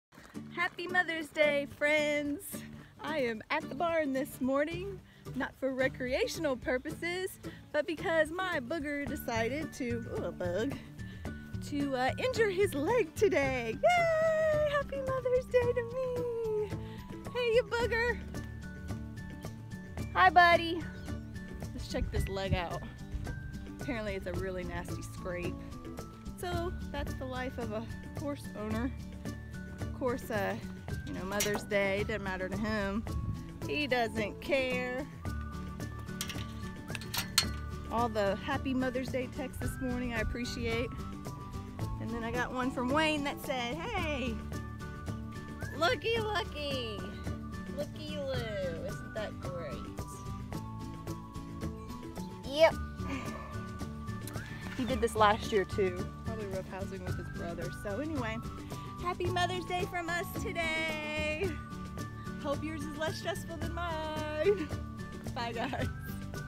Leave it to MY horse to do this on Mother’s Day! 🤦🏻‍♀️🐴😅
Happy Mother’s Day to all the women out there that hustle daily, love and nurture always, and just handle business when it needs to get done. Patriot decided to somehow scrape his leg up pretty bad, so I had to wash it and put disinfectant on it. Oh boy. But ground work begins this week...finally!! 🙌🏼🐴🐴💕